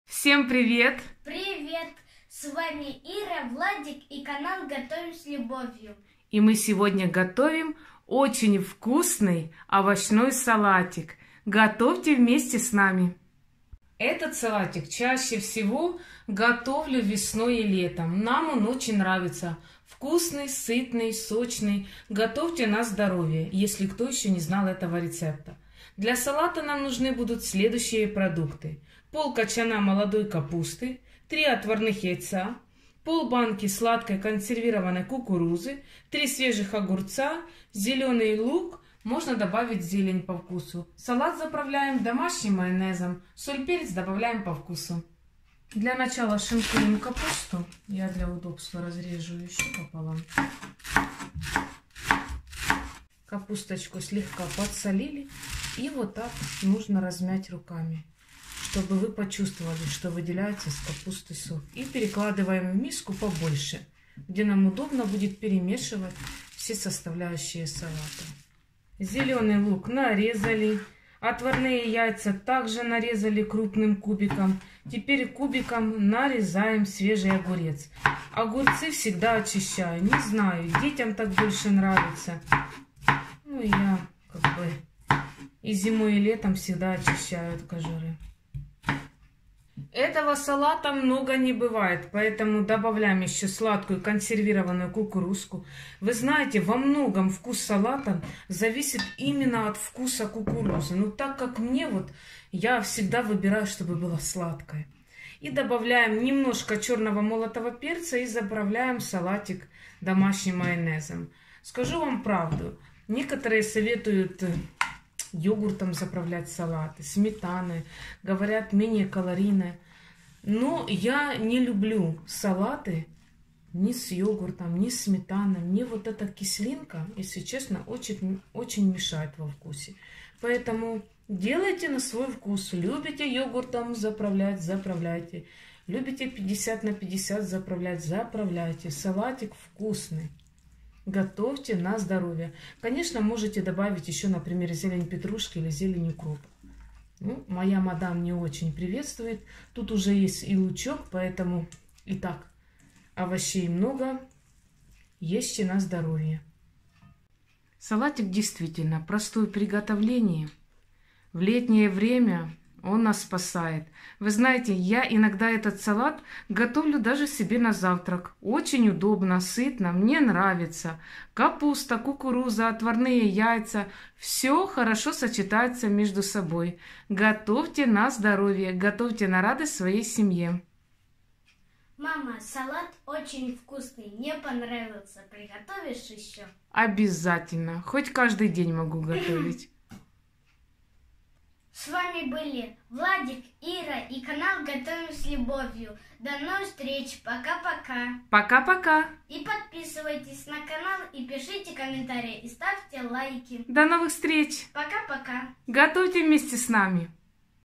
Салат из молодой капусты Вкусный летний салат
Канал ГОТОВИМ С ЛЮБОВЬЮ на Яндекс Дзен ссылка для перехода -

0,5 кочана капусты
3 отварных яйца
0,5 б. консервированной кукурузы
3 свежих огурца
зеленый лук
домашний майонез

🍞ПАСХАЛЬНЫЙ кулич ( ПАСКА) Лучшие ПРОВЕРЕННЫЕ рецепты на ПАСХУ